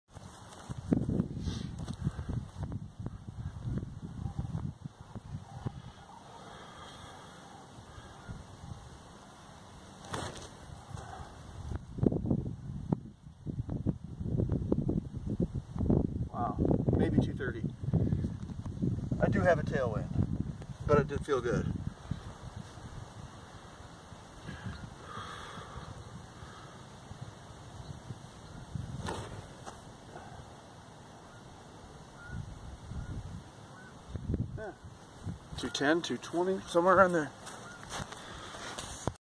Disc Golf Form Critique - Putter Standstill, Pt. 3
Hooray, a breakthrough this evening!  I'm somewhere around 1,500 total putter standstills since starting this season and only on my third session after adopting Sidewinder22's pendulum-style standstill from DGCR.

I threw probably 100.  Then I filmed two throws (not these) and watched it on the spot.  I grew irritated with how slowly and lazily my delivery always looks so I decided to get a little pissed about it and add some power.

I know I've got to keep my arm loose for this, not tense, and same for my core and hips and legs.  I focused on that and made myself RIP it.

The first throw here (a dyed Fuzion Burst Deputy) went 93 paces.  Every 11 of my paces is about 30', so 88 paces is 240' and that makes this a 250+' toss. (Yes, with a tail wind, but it still felt awesome).  The next (a dyed Gold Line Gauntlet) went 85 paces.  The best-ever and also a top ten throw for me all year, back to back.

I never in my life have thrown a putter that far, and I did it here with a standstill.  My Rocs and Buzzes with a full x-step weren't going this far as recently as last season.

I'm stoked.  And I made sure I threw another 50 or 60 before I called it quits due to darkness so I wouldn't forget what all my muscles are supposed to do.  I'd have filmed more, but my stupid phone thinks it's out of battery power after about three minutes of filming or watching even though it says 75% when I get back in the house and plug it in.  Whatever.  Technology is great...when it works.

Anyway, I'm looking forward to when this is my average putter standstill, not my best ever.

(Weather = 31 degrees F with a stiff 15 mph tail wind.  Wind chill of 21 degrees F.)